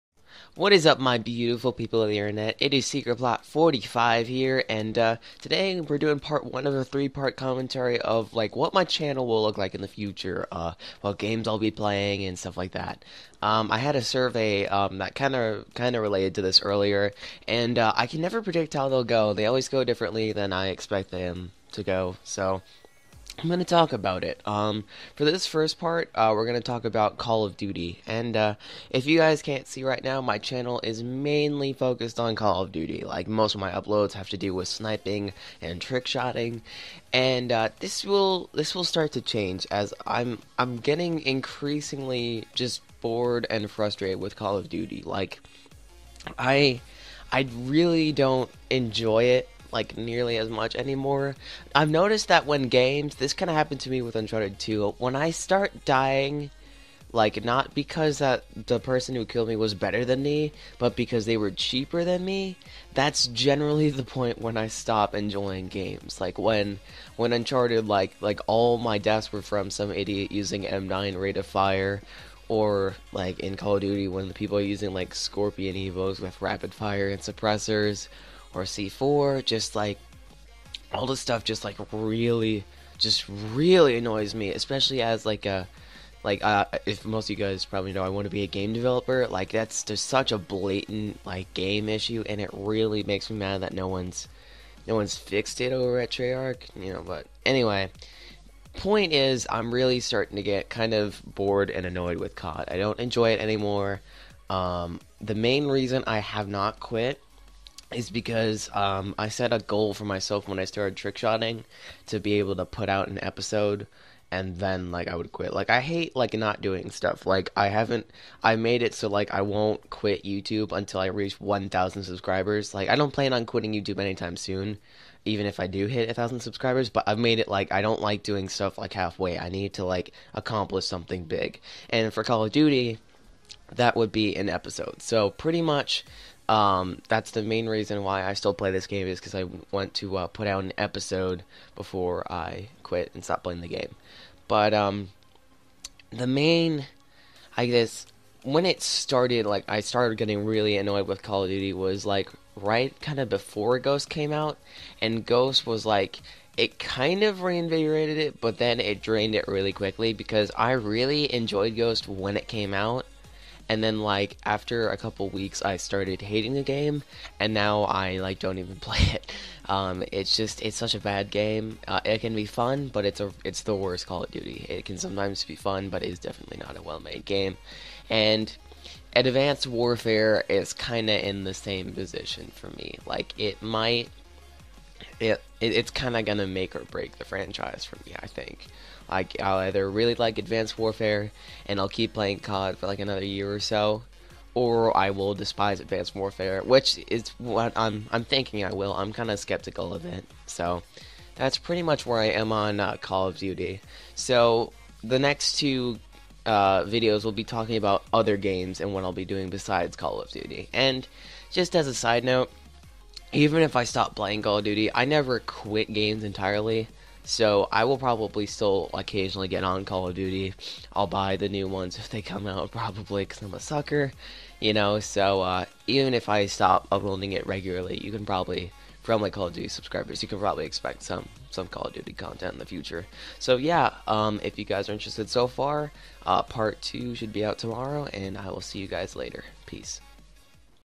The Future of Secretplot45! - Part 1: CoD
This is a 3 part series where I'll go over what games and content I'll be bringing to you in the near future! First up: Call of Duty.

Steam: Secretplot45